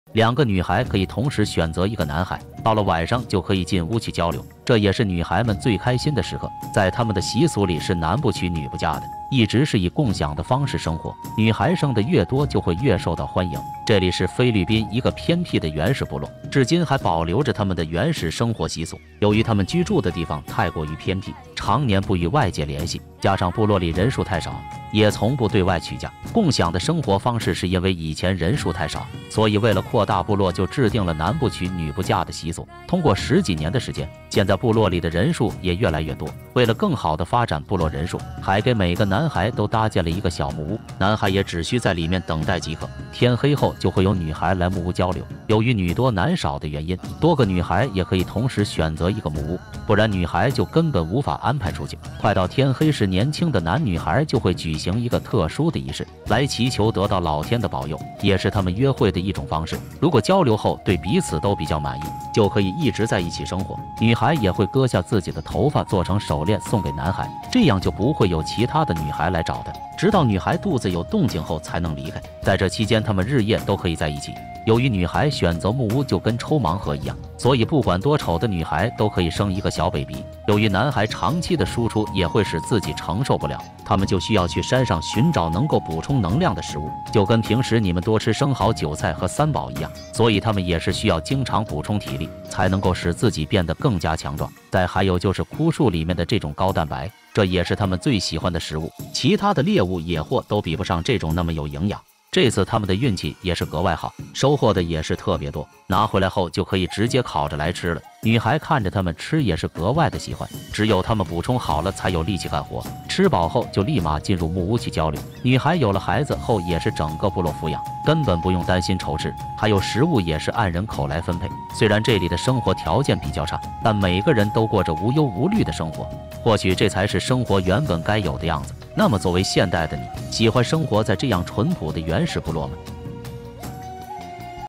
原始部落婚姻習俗，女孩可以選擇任何男人生孩子，大家以共享方式生活，真實紀錄片
原始部落婚姻習俗